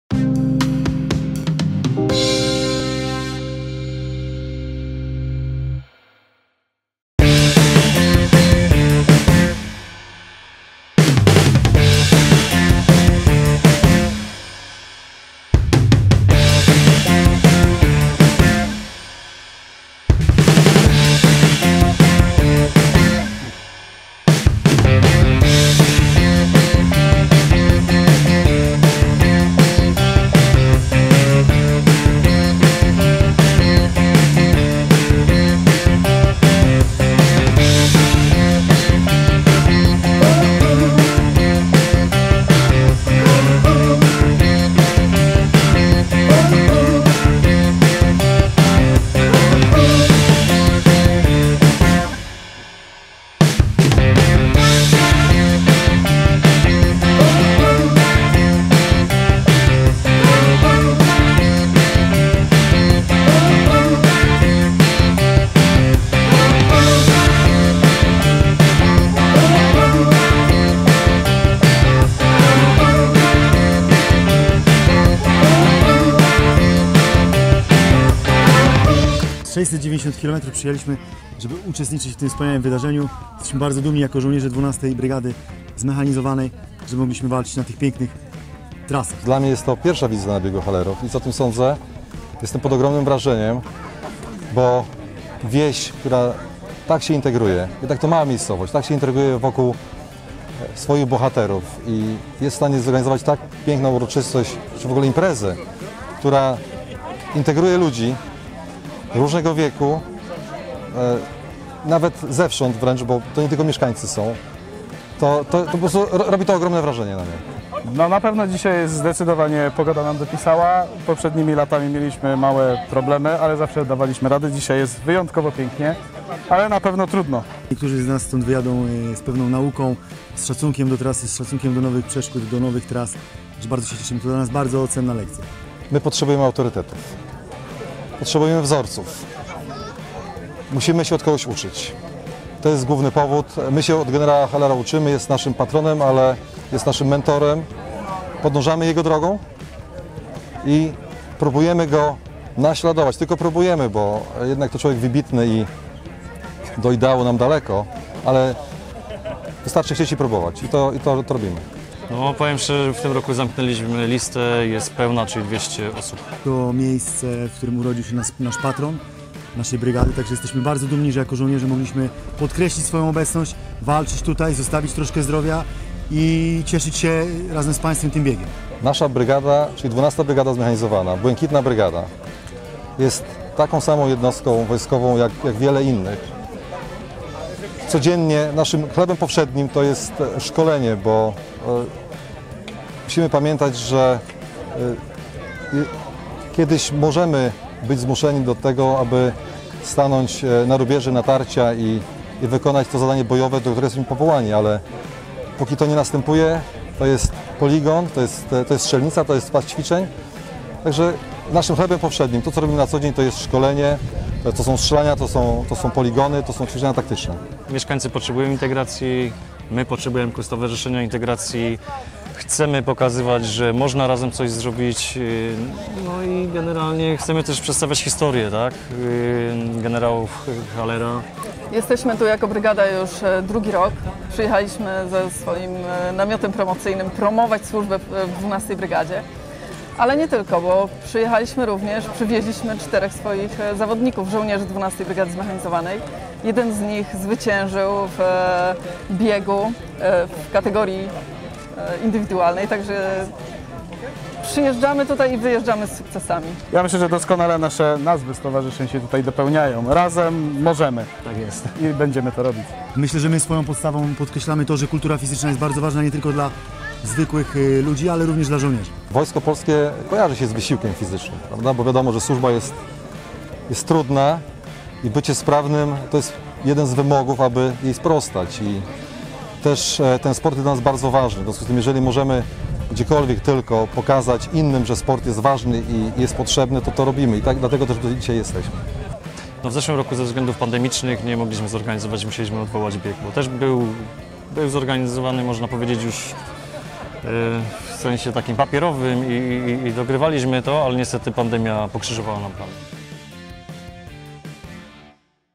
IV Bieg Hallerów za nami!
19 czerwca odbył się „IV Bieg Hallerów" - bieg terenowy z Polanki Hallera do Jurczyc. Zawodnicy wystartowali z Polanki Hallera, gdzie znajduje się dwór generała Stanisława Hallera i pobiegli do znajdującego się w Jurczycach dworu generała Józefa Hallera. Organizatorami wydarzenia były: Stowarzyszenie Razem dla Wszystkich w Jurczycach i Stowarzyszenie Możemy z Polanki Hallera.